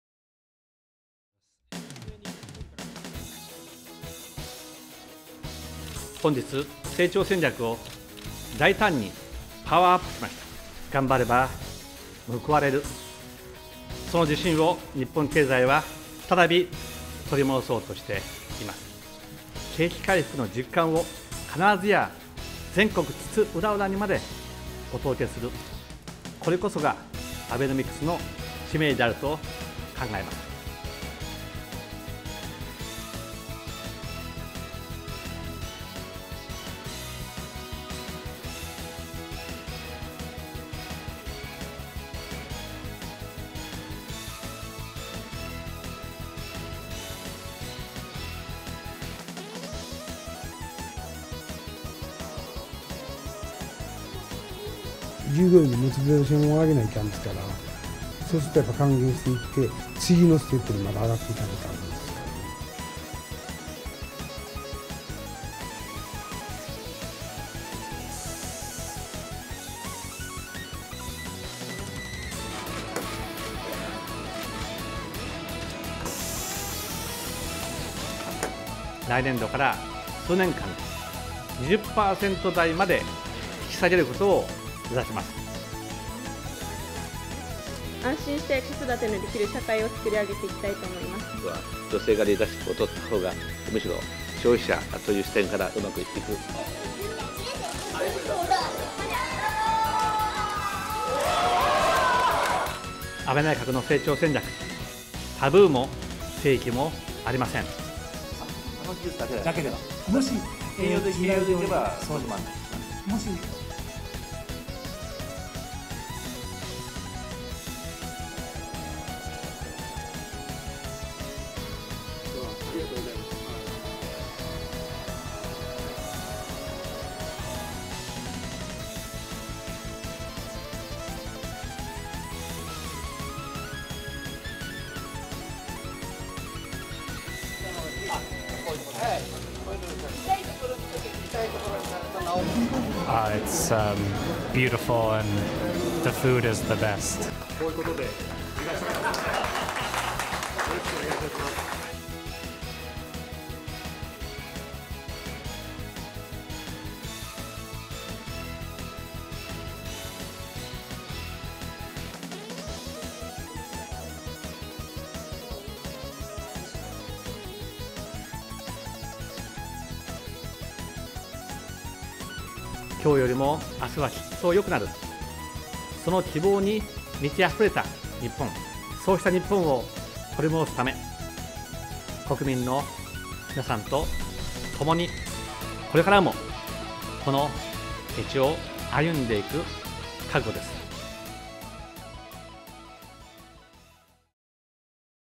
安倍総理 成長戦略2014 －平成26年6月25日
新たな成長戦略の公表にあたって、国民の皆さまへ向けた動画(3分半)です。